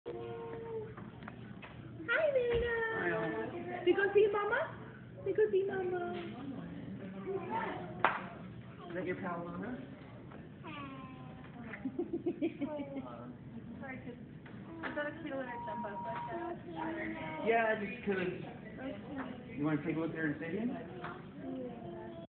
Make a Wish Puppy Reunited with Best Friend After Surgery
Because of you, Luna, the puppy Ava received as her wish from the Make a Wish Foundation, gets to go home! Luna received lifesaving surgery after ingesting the head of a teddy bear, thanks to everyone that donated to Frankie's Friends!